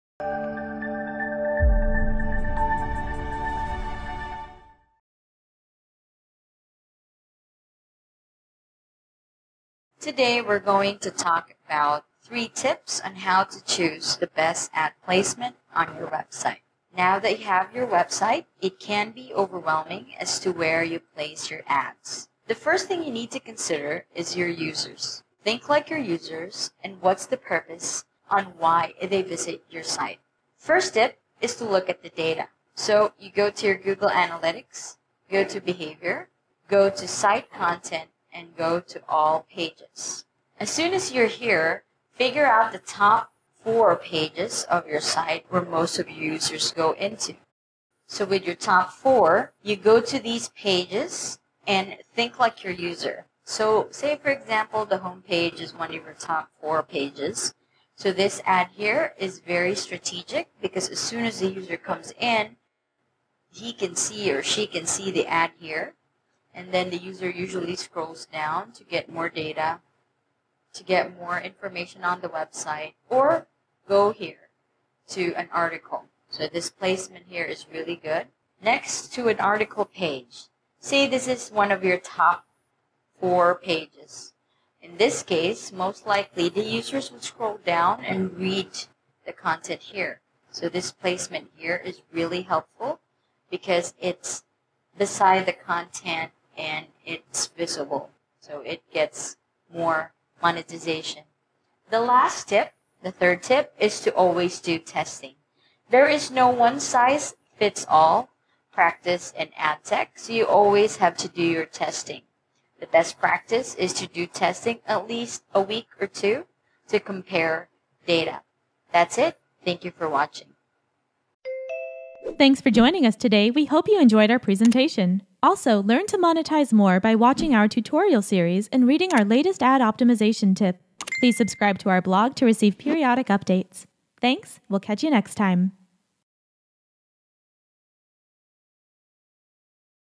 3 Tips on How to Choose the Best Ad Placement on your Website
Need help optimizing your ad inventory and increasing ad revenue? Sign up to MonetizeMore today!